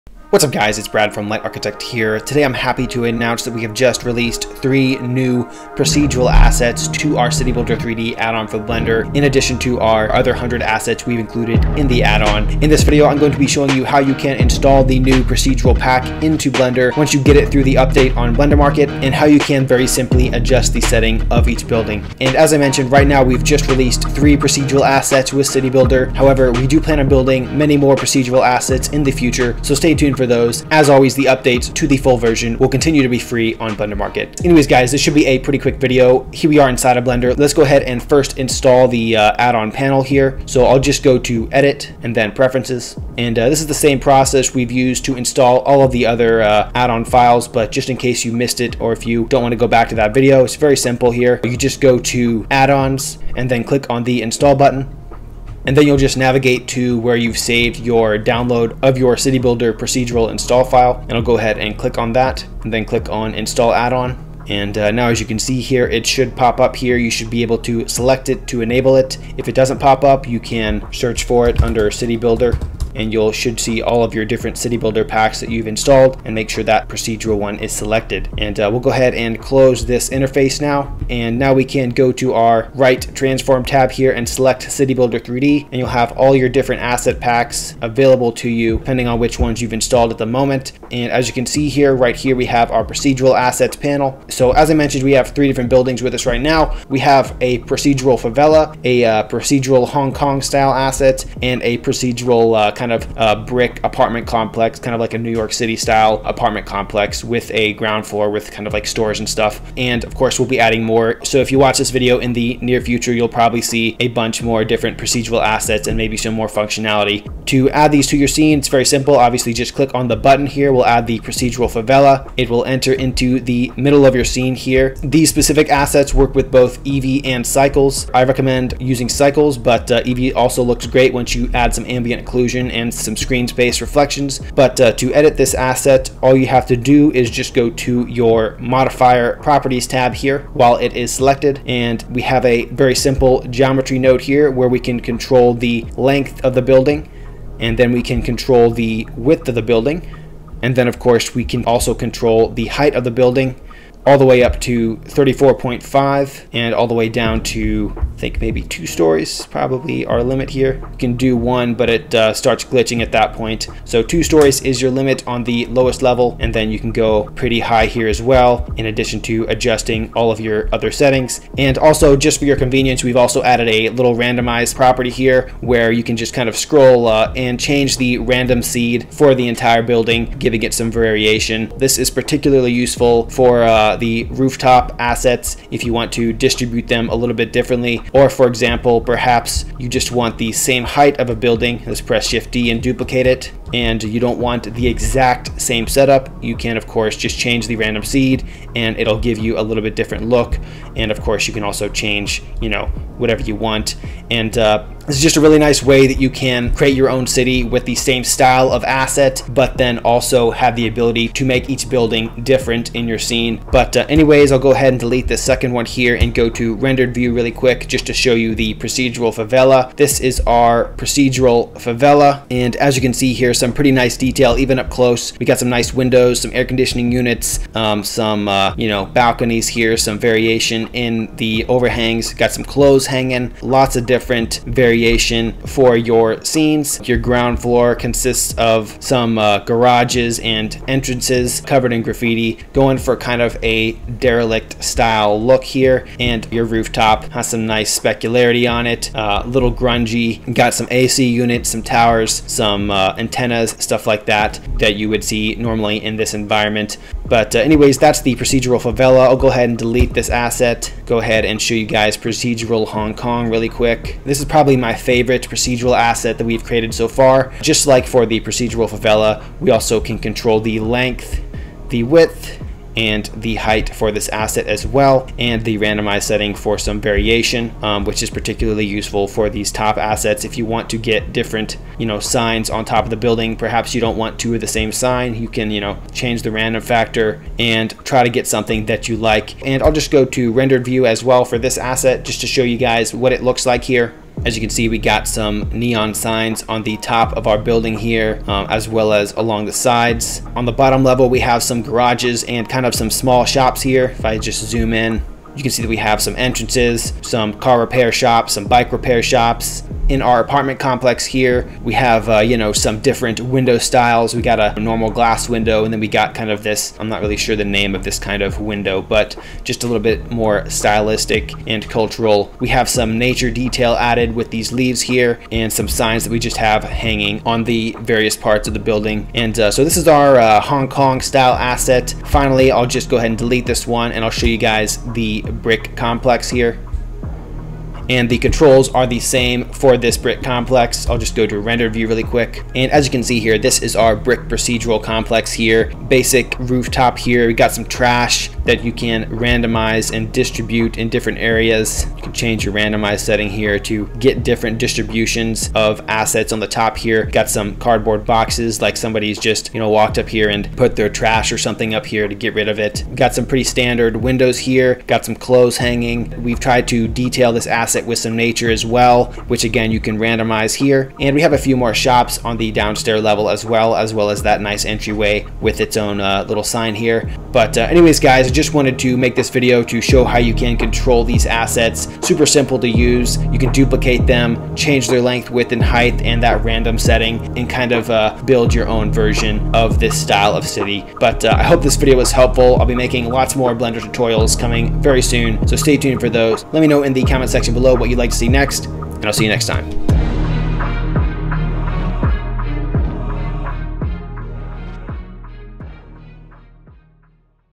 CityBuilder3d Add-on Announcement: New Procedural Buildings for Blender(UPDATED for Blender 3.2!)
Hey Everyone! This is a short announcement and demo video of some of the new 3d procedural assets we've recently released in our CityBuilder3d add-on for Blender. More tutorials are coming very soon!